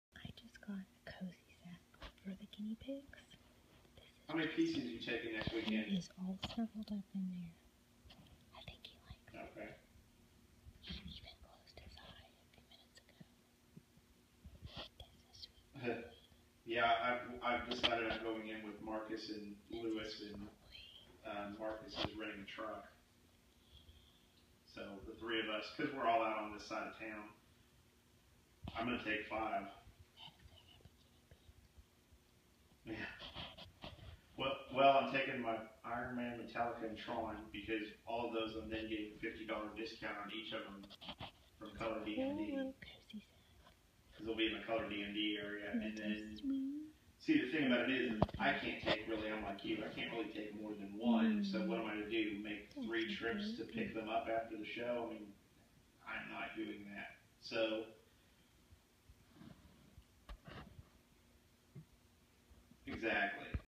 3 11 16 Sleepy Chester
This is my sweet guinea pig, Chester!  He was in my classroom for a few years! We all adored them so much!